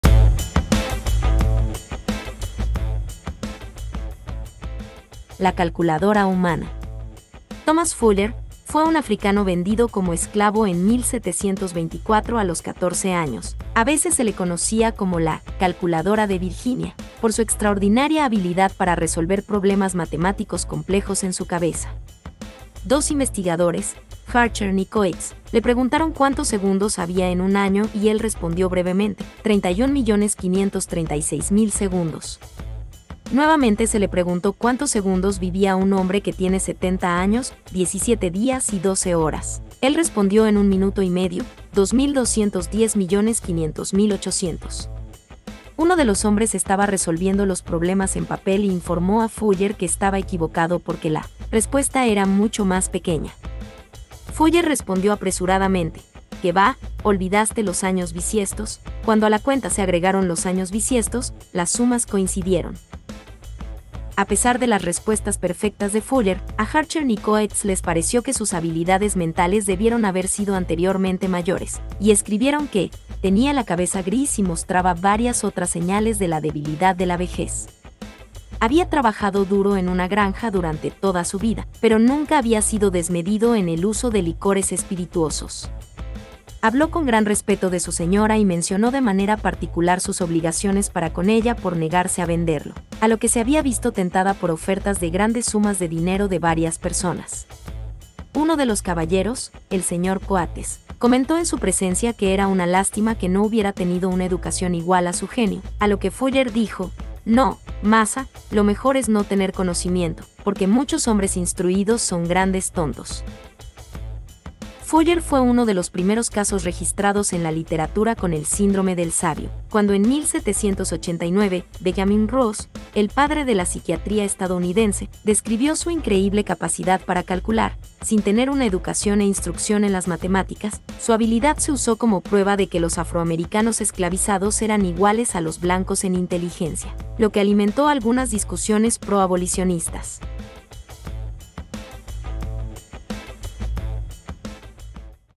Hombre Calculadora Thomas Fuller (era un esclavo analfabeta)
El Hombre Calculadora Thomas Fuller fue un esclavo analfabeta.
Fuller fue uno de los primeros casos registrados en la literatura con el síndrome del sabio, cuando en 1789, Benjamin Rush, el padre de la psiquiatría estadounidense, describió su increíble capacidad para calcular, sin tener una educación e instrucción en las  matemáticas, su habilidad se usó como prueba de que los afroamericanos esclavizados eran iguales a los blancos en inteligencia, lo que alimentó algunas discusiones pro-abolicionistas.